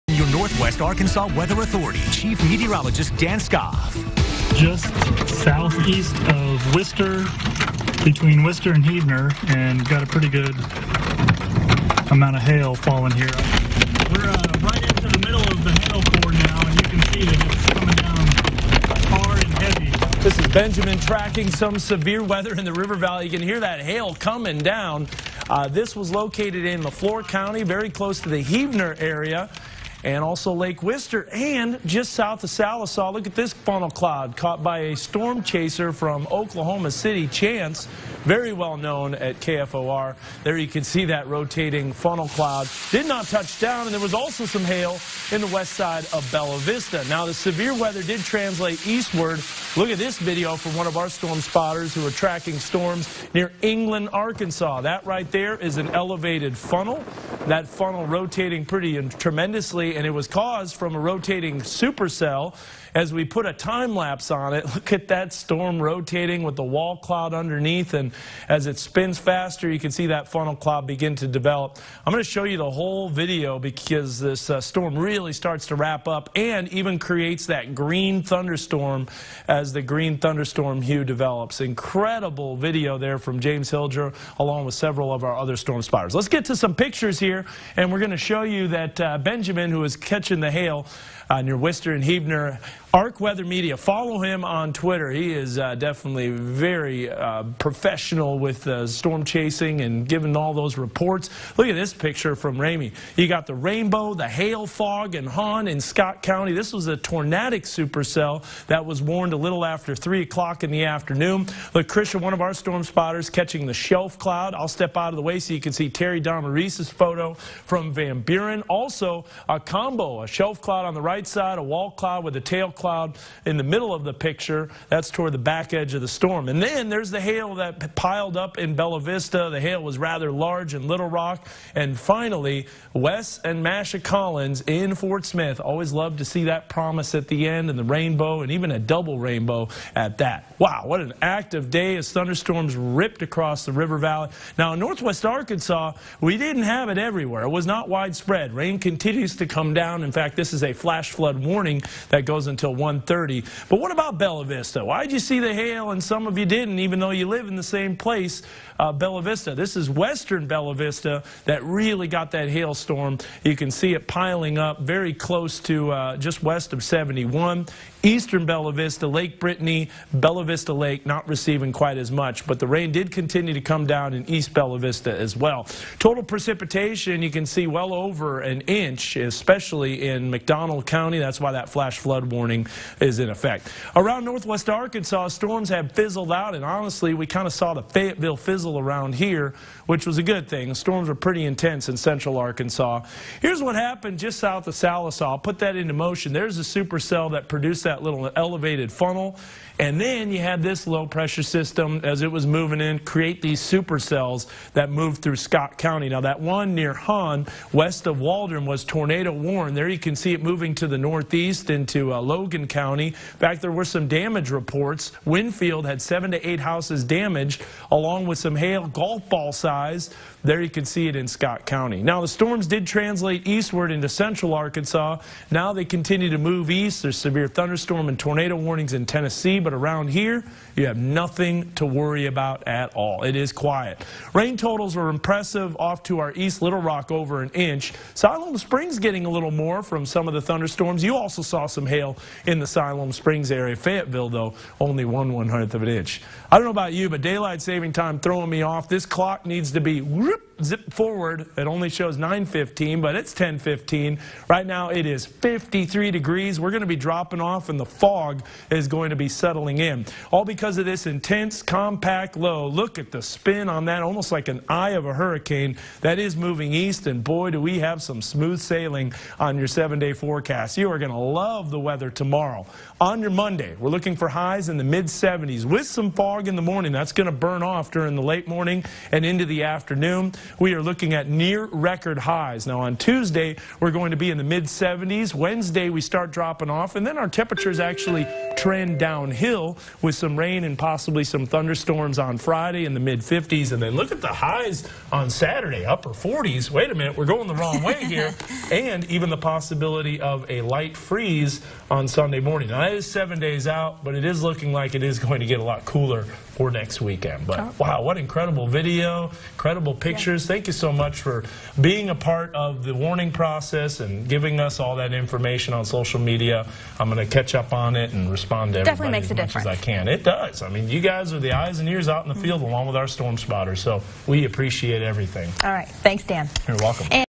EMMY 2016 | Cat. 42 News Weathercast KNWA
On March 13th this year, a large weather system brought severe weather to parts of the viewing area.  This is an extended weathercast during the 10 PM news to showcase the captivating weather video, amazing time-lapse photography and unique pictures our weather team received through social media.  I started the weathercast with a cold open & hail nats to grab the viewers attention.  Then I featured some pictures we received on social media followed by a recap of the event, including a hyper-local view of hail only on one side of Bella Vista.  Then I moved into the forecast, which was rather quiet after this severe weather event.